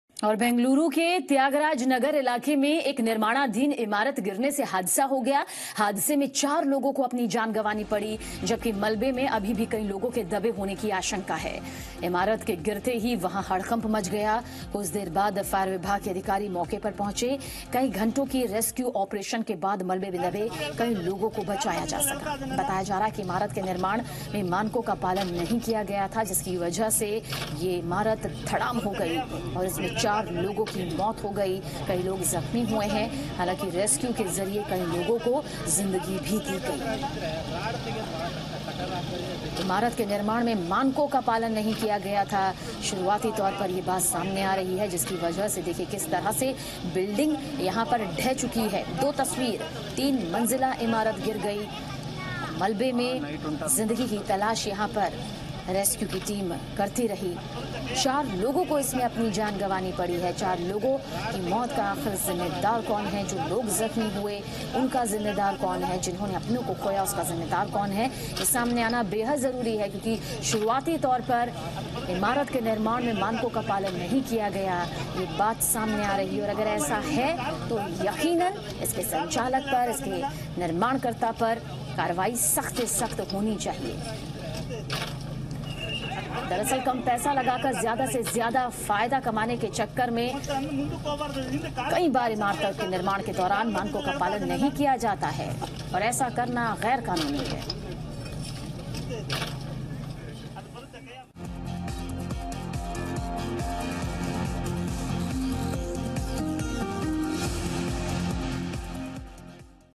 बेंगलुरु: निर्माणाधीन इमारत गिरने से हुआ हादसा, 4 लोगों की मौत
बेंगलुरु के त्यागराज नगर इलाके में एक निर्माणाधीन इमारत गिरने से हादसा हो गया. हादसे में 4 लोगों की मौत हुई हैं. जबकि मलबे में अभी भी कई लोगों के दबे होने की आशंका है.